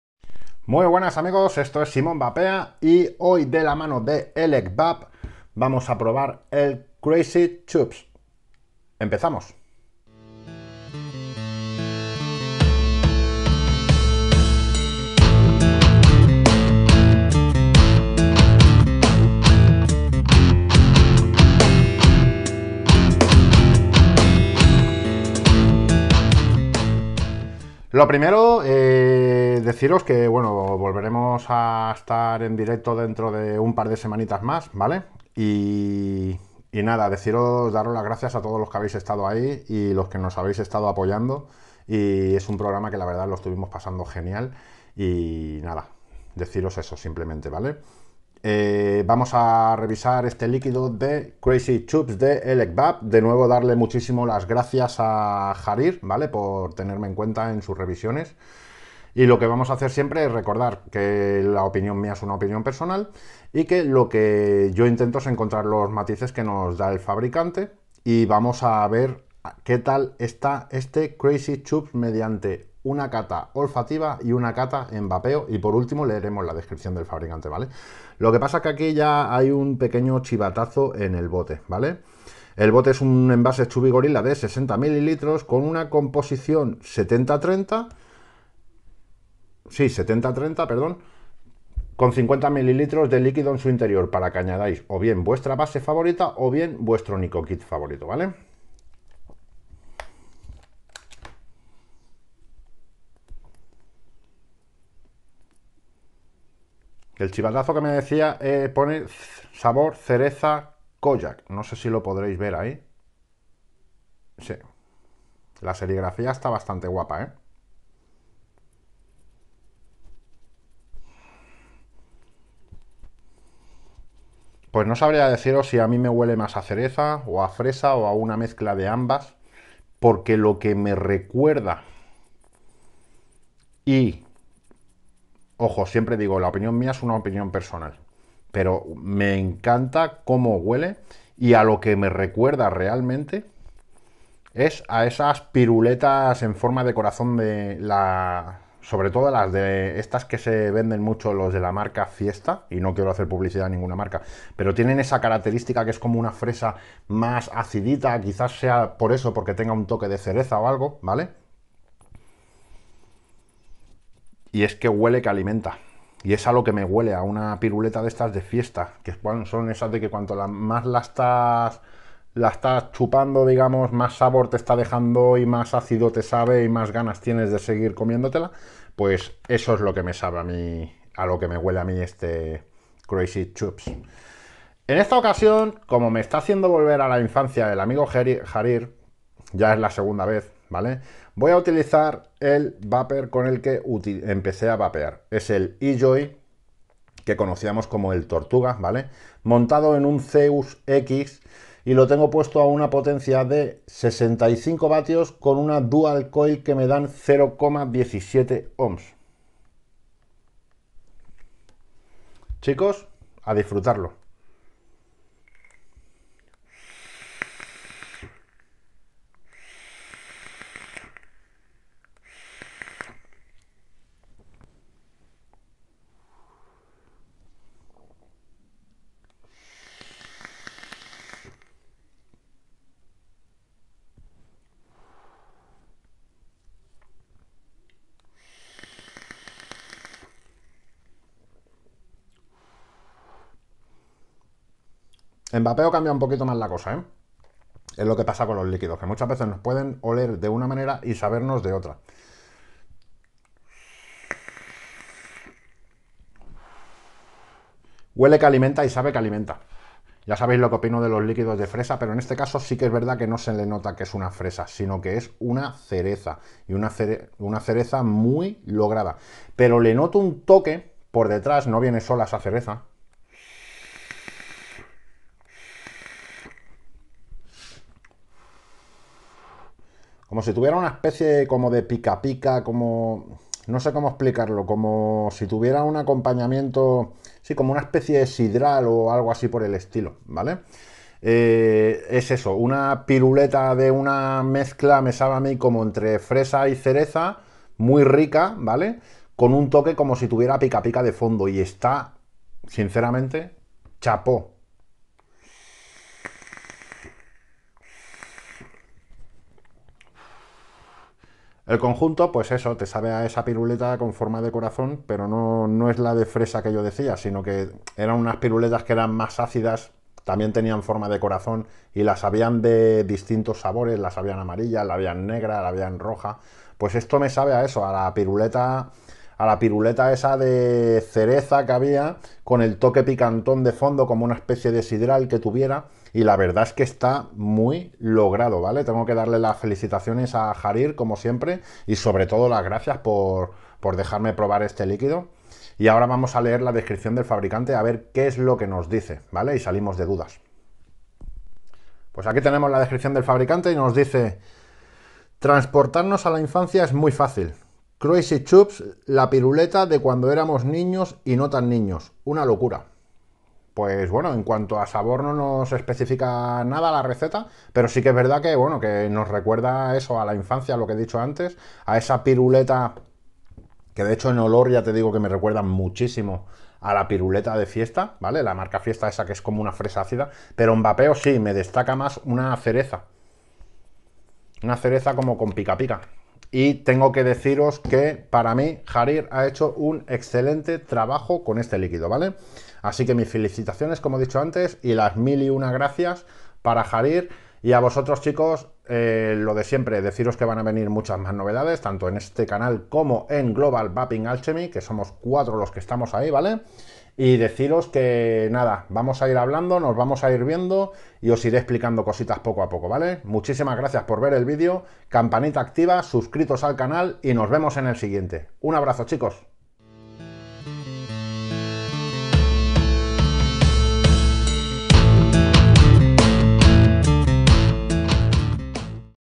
Crazy Chups de Elec-Vap... Me volvió a la infancia de nuevo!!
Trasportarnos a la infancia es muy facil , CRAZY CHUP la piruleta de cuando eramos niños y no tan niños. Una locura.